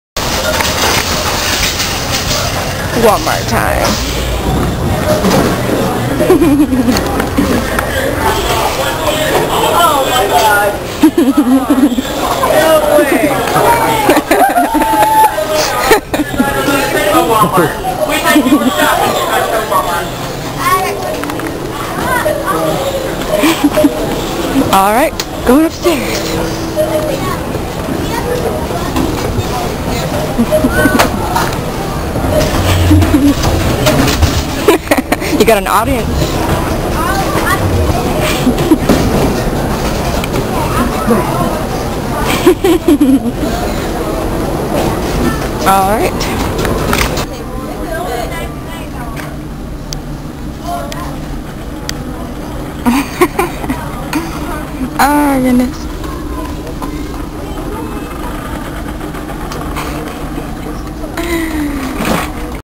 Furries at Wally World
fursuiting at walmart lol.

"OMG NO WAY!!"

i stole the Furaffinity raccoon's tie ^^ lol